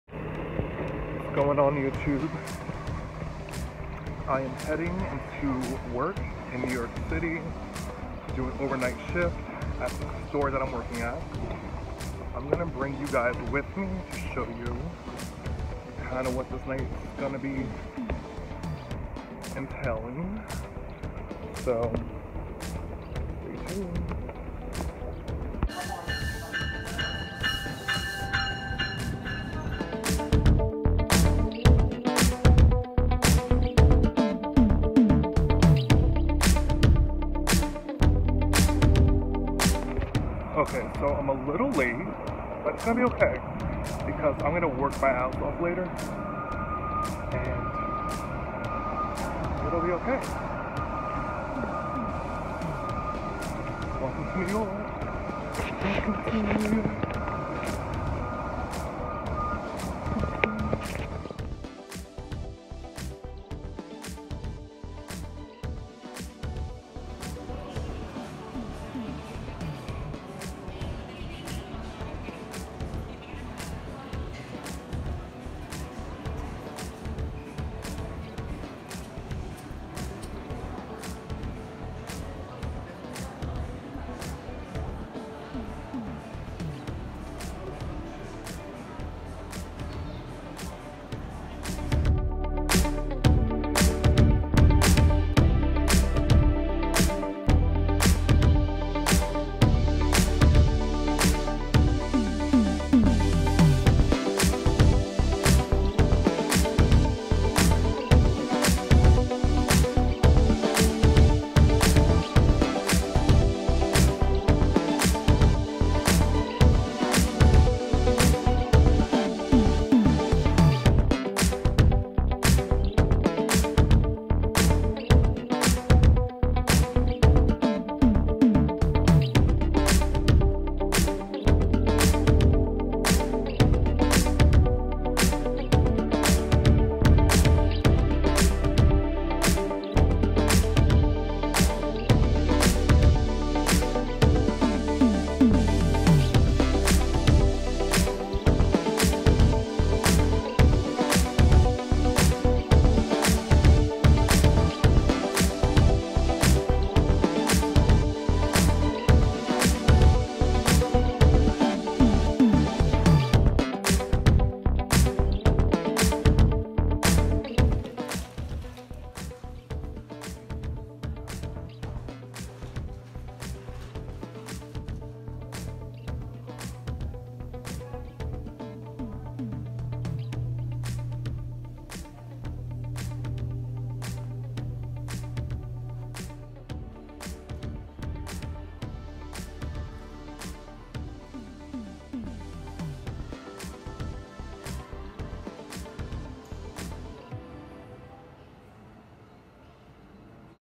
Luxury Fashion Brand Visual Merchandising Work Vlog
Luxury Fashion Visual Merchandising
Setting up & dressing mannequins
Store Displays
Luxury items